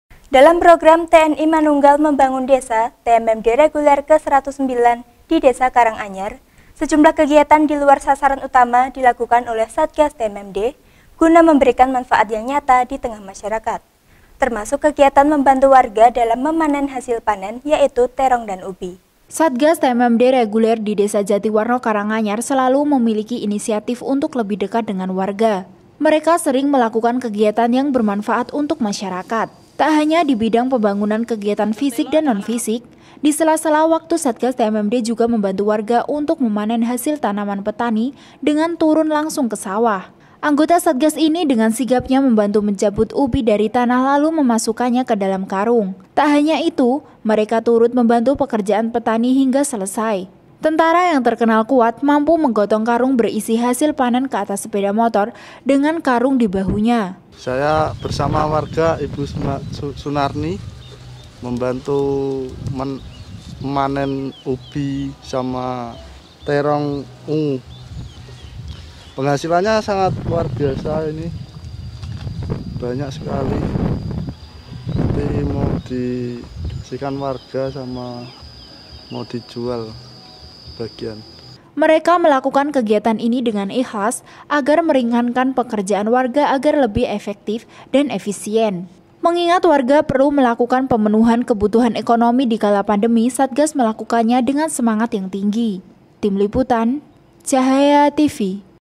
Satgas TMMD Bantu Warga Panen Terong Dan Ubi
Dalam program TNI manunggal membangun desa (TMMD) Reguler ke-109 di Desa Karanganyar, sejumlah kegiatan diluar sasaran utama dilakukan oleh Satgas TMMD, guna memberikan manfaat yang nyata ditengah masyarakat. Termasuk kegiatan membantu warga dalam memanen hasil panen yaitu teorng dan ubi.